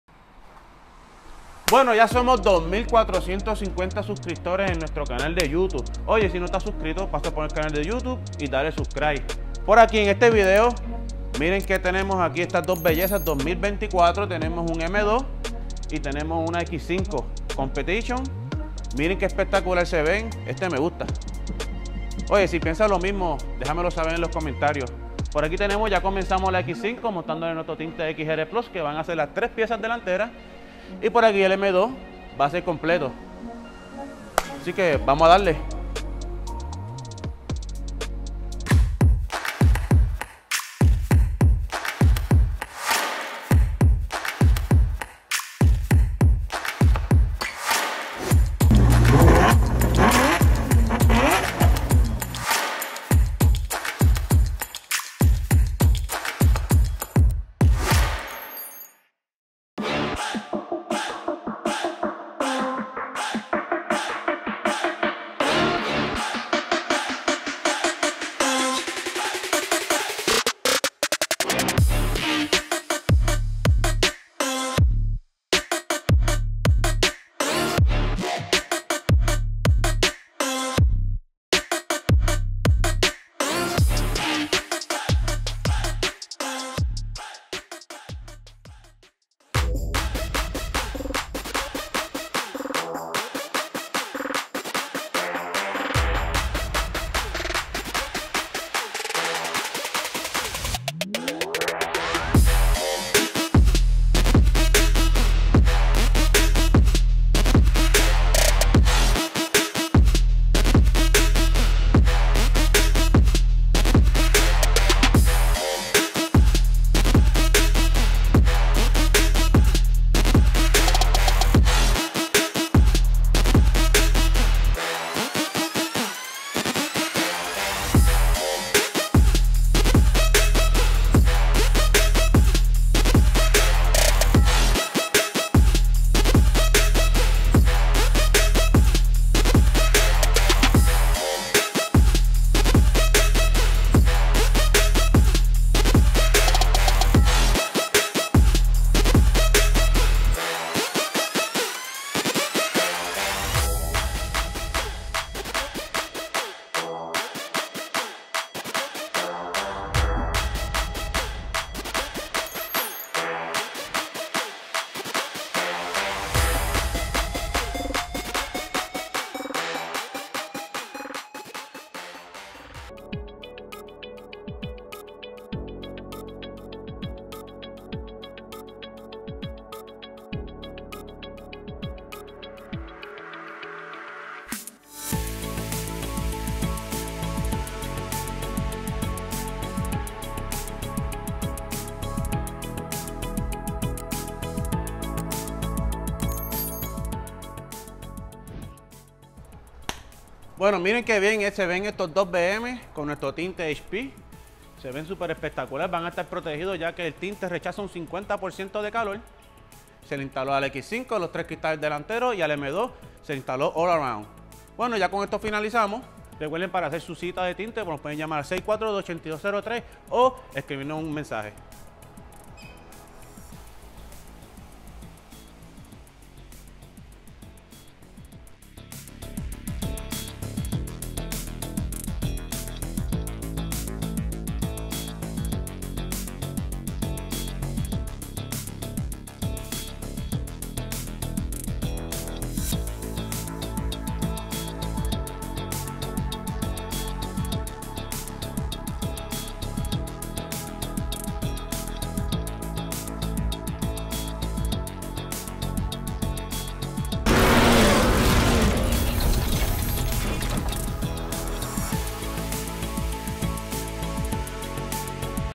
Ep33: X5 y M2 | Xpel Prime HP
M2 y X5 Competition para nuestra mejor proteccion solar Xpel Prime HP
Mas informacion o cotizacion